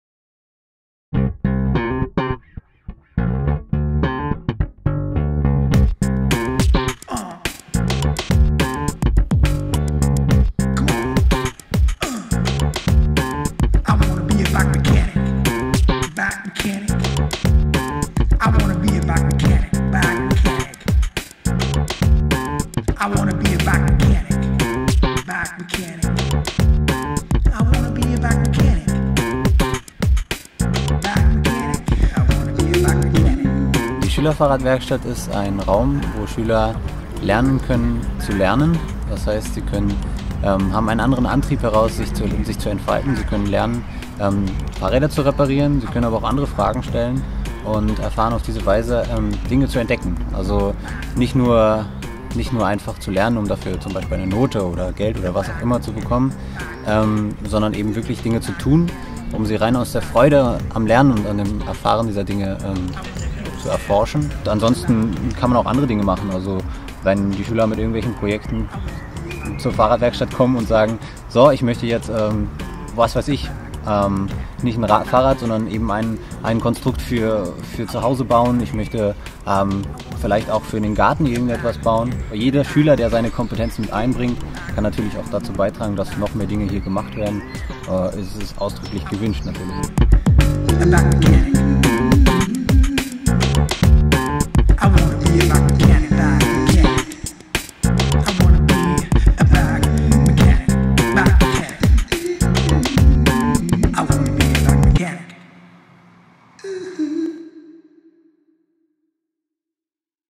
Die Schüler-Fahrrad-Werkstatt Berlin-Dahlem
Die Schüler-Fahrrad-Werkstatt ist eine Schülerinitiative der Rudolf-Steiner-Schule in Berlin-Dahlem.

→ Hier geht es darum zu erfahren, wie Lernen aus innerer Motivation heraus, die Potentiale beflügelt.

Das Fahrrad ist hier nur ein Teil der Initiative!